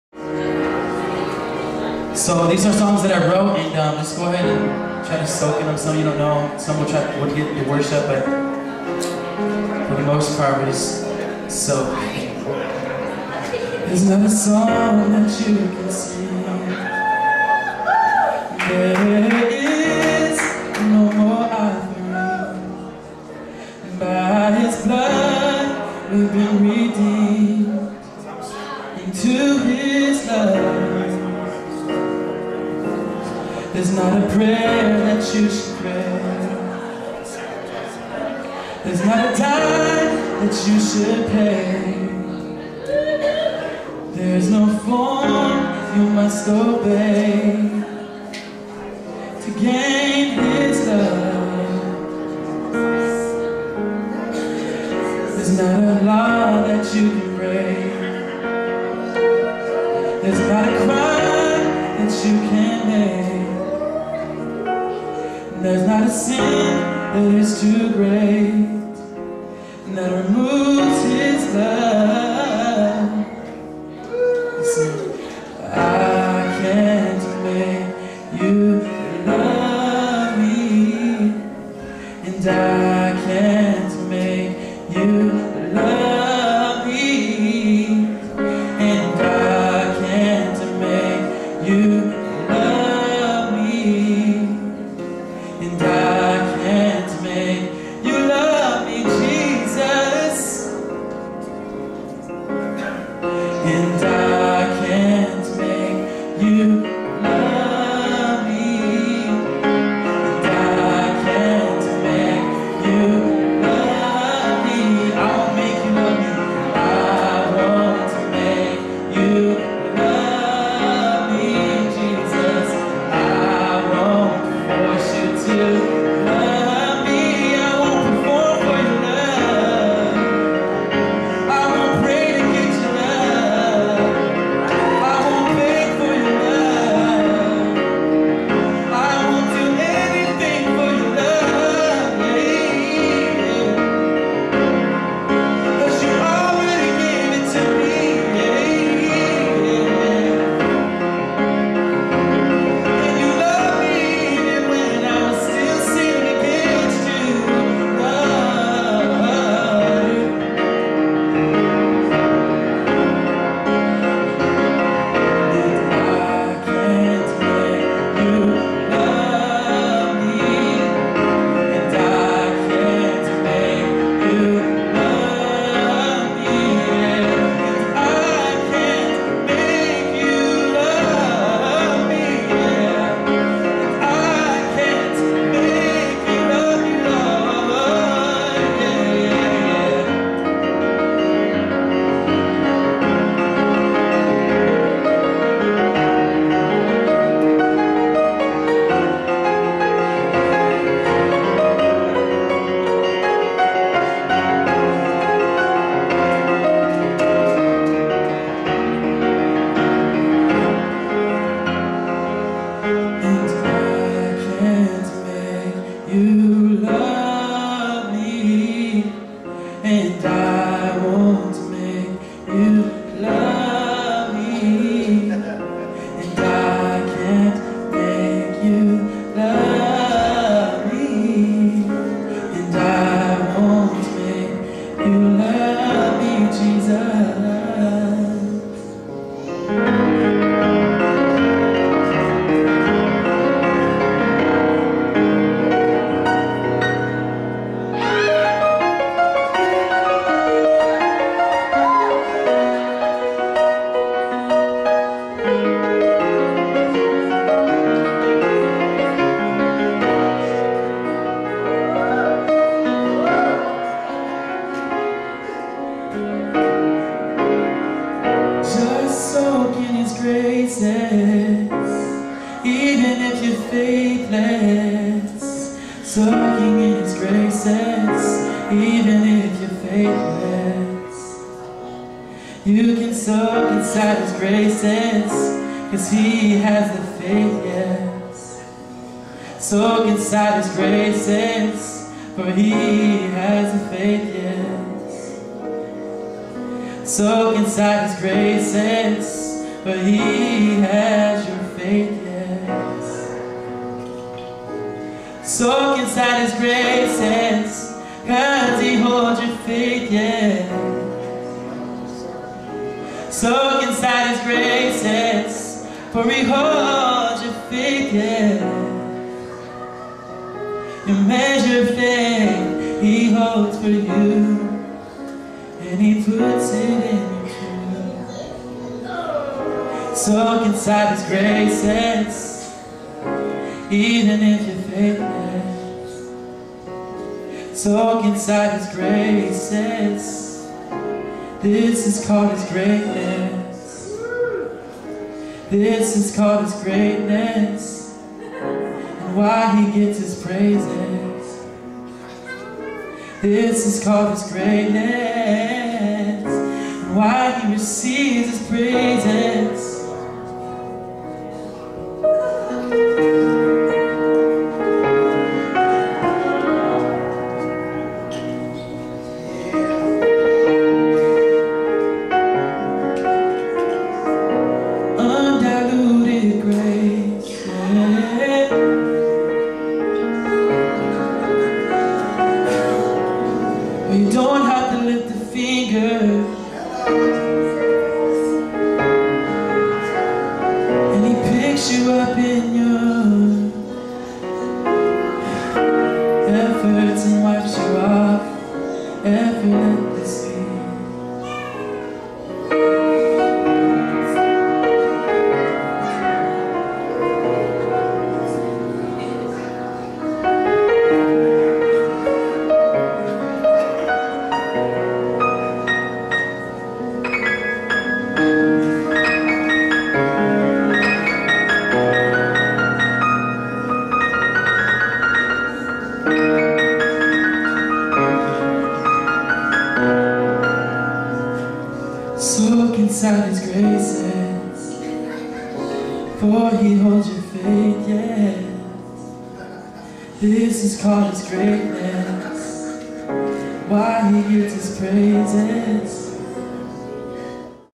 Sausalito # 1 Erik Perales
Erik Perales on piano at Sausalito Christian Fellowship July 15, 2011.  Erik is worship leader in Tracy, CA at The Glory Service.
Originally this was not recorded for video purposes, but the music on this recording is just so outstanding I had to share it with you guys!  Erik just finished recording his first album and it should be out later this year.  In the meantime, sit back and enjoy the gift of God in Erik Perales!